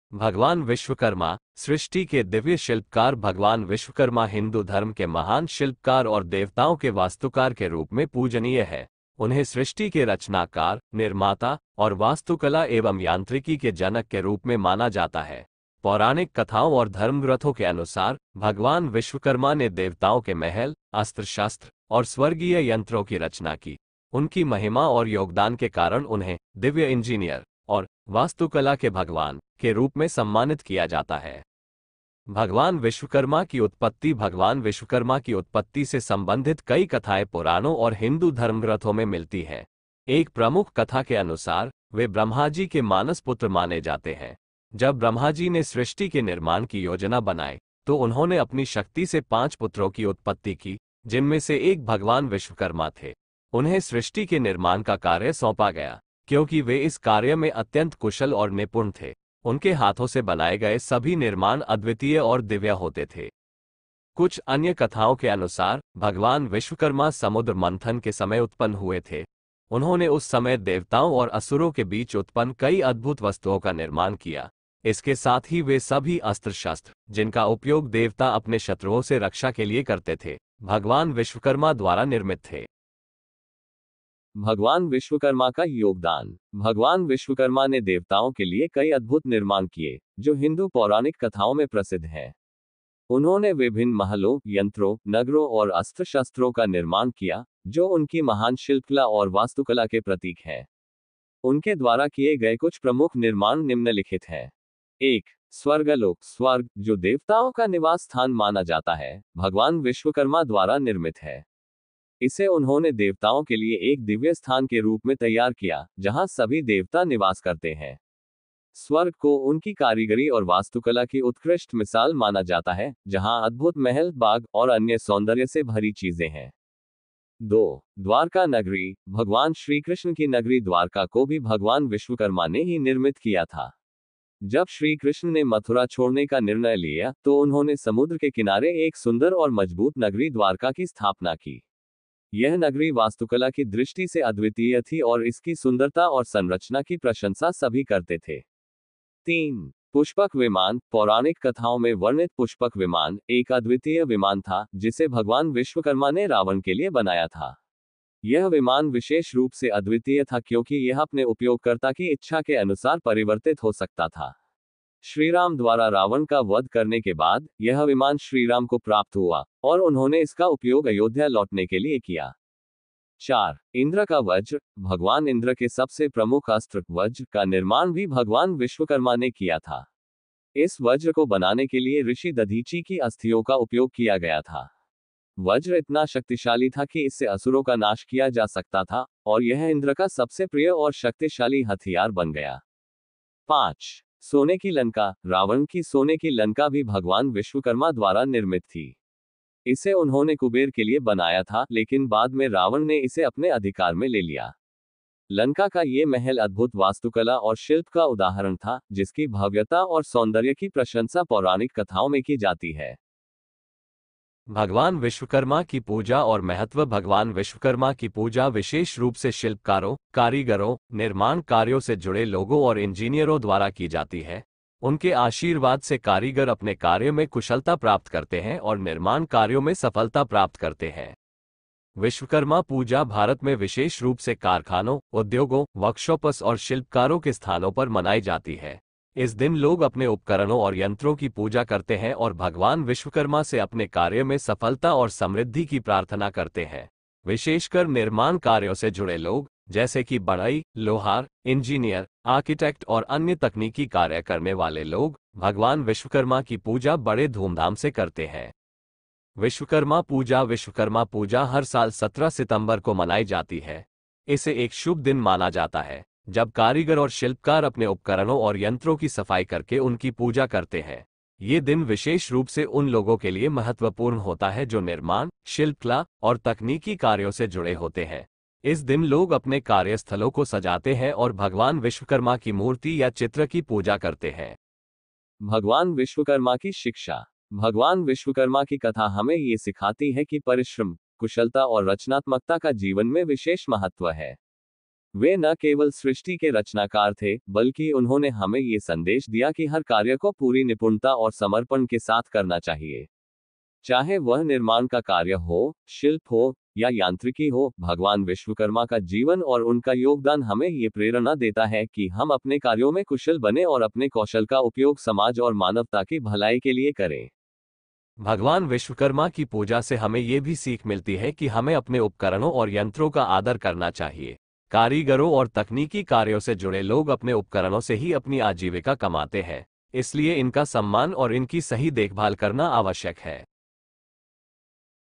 Vishvakarma (भगवान विश्वकर्मा)
भगवान विश्वकर्मा को हिंदू धर्म में सृष्टि के महान शिल्पकार और वास्तुकार के रूप में पूजा जाता है। उनकी महान कारीगरी और निर्माण कार्यों की महिमा ने उन्हें देवताओं का प्रमुख वास्तुकार और इंजीनियर बना दिया। उनके द्वारा निर्मित स्वर्गलोक, द्वारका, पुष्पक विमान, इंद्र का वज्र और सोने की लंका उनकी अद्वितीय शिल्पकला और वास्तुकला के प्रमाण हैं।

भगवान विश्वकर्मा की पूजा और आशीर्वाद मुख्य रूप से उन जातियों और समुदायों द्वारा प्राप्त किया जाता है जो निर्माण, शिल्प, कारीगरी और तकनीकी कार्यों से जुड़े होते हैं। इनमें प्रमुख रूप से बढ़ई, लोहार, स्वर्णकार (सोने-चांदी के कारीगर), मूर्तिकार, राजमिस्त्री और अन्य कारीगर समुदाय शामिल होते हैं।
विश्वकर्मा को शिल्प और निर्माण के देवता माना जाता है, इसलिए उनके आशीर्वाद के लिए इन समुदायों में विशेष श्रद्धा होती है। विश्वकर्मा पूजा का आयोजन विशेष रूप से भारत के विभिन्न हिस्सों में उद्योगों, कारखानों, वर्कशॉप और कारीगरों के स्थानों पर किया जाता है, जहाँ सभी अपने उपकरणों की पूजा कर भगवान विश्वकर्मा से समृद्धि और कुशलता की प्रार्थना करते हैं।